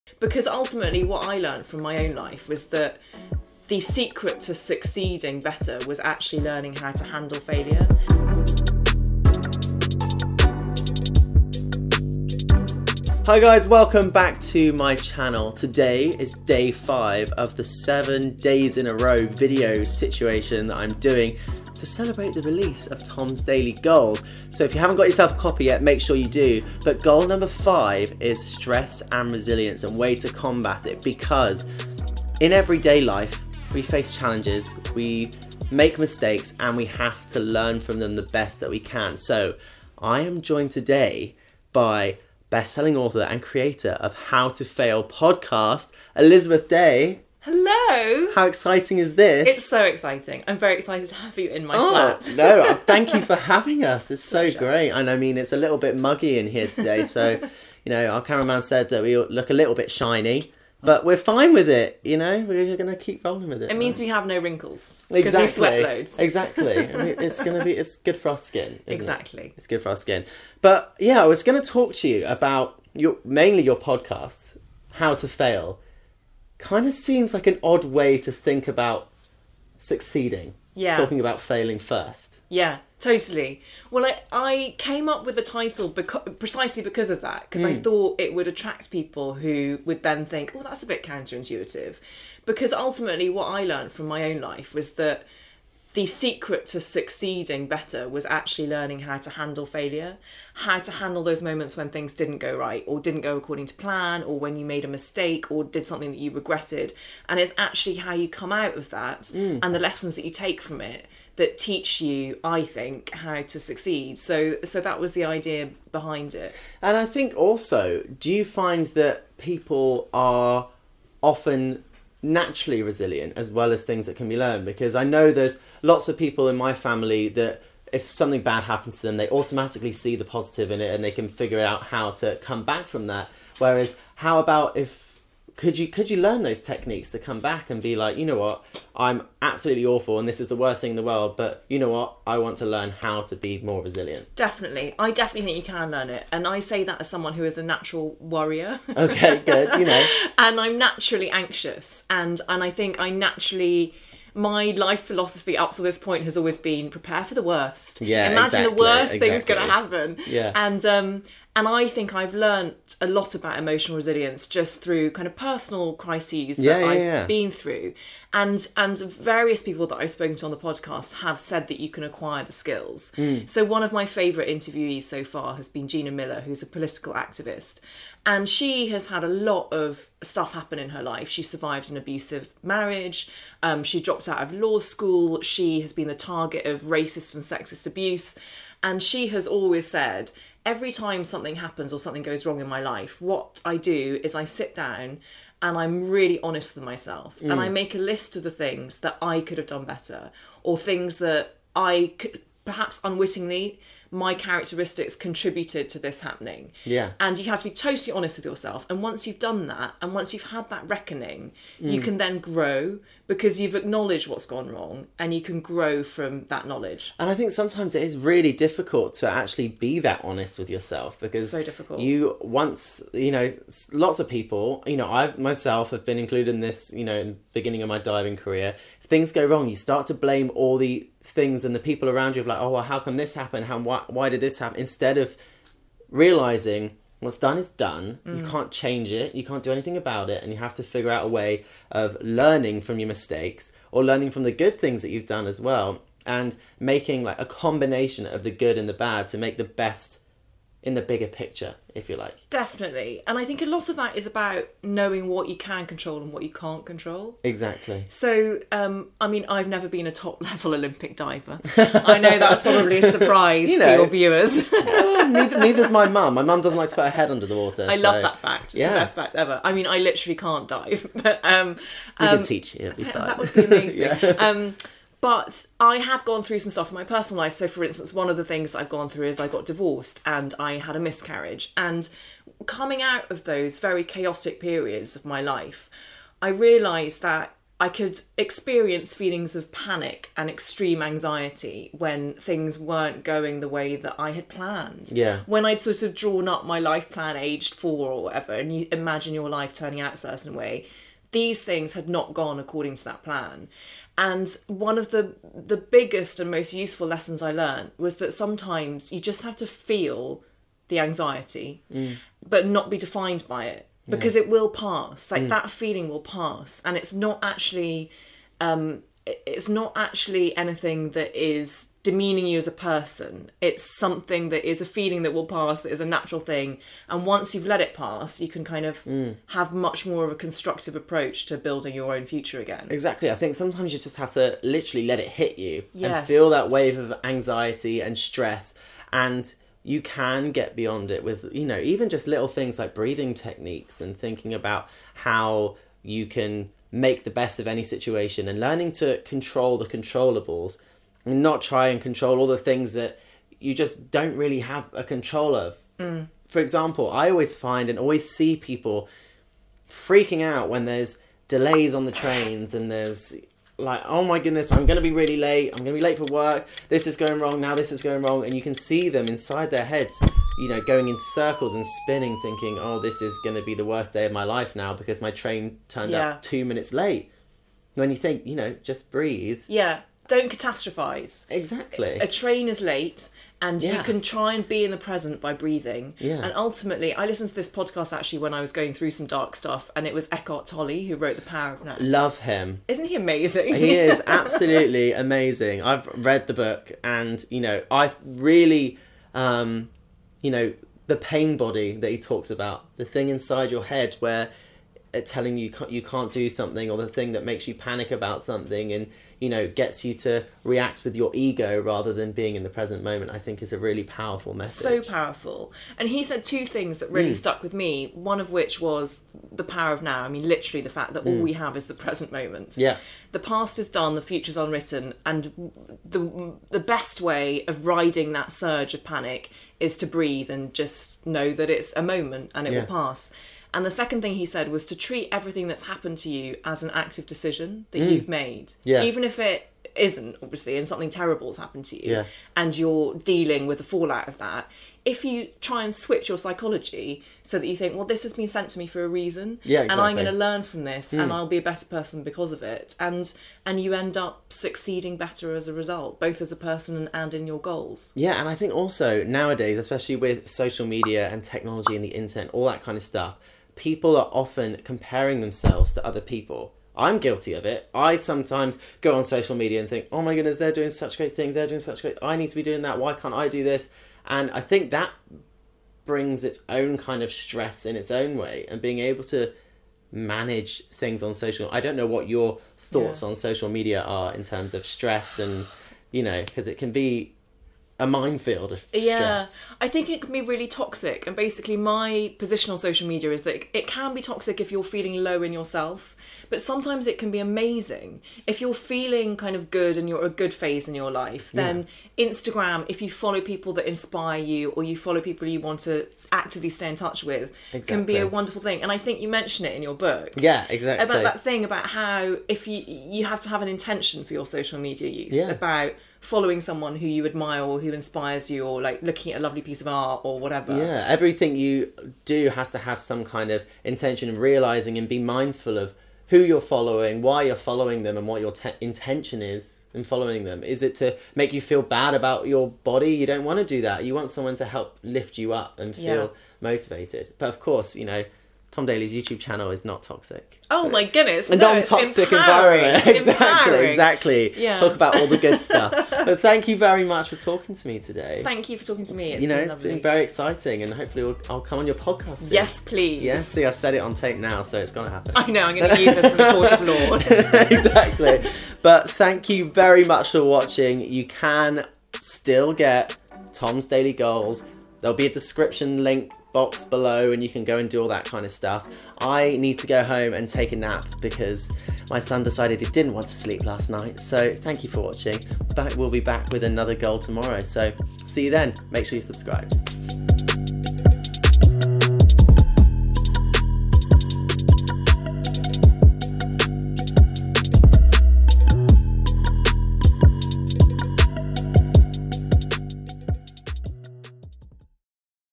Learn How To Fail Ft. Elizabeth Day *GOAL 5* I Tom Daley
Goal 5 in Tom's Daily Goals is STRESS AND RESILIENCE and I had a chat with Elizabeth Day, best selling author and has the podcast Learn How To Fail!  Being resilient is something that comes natural to some people, but it also can be learned!

Stress can hit people in different ways and the same with resilience, how resilient are you?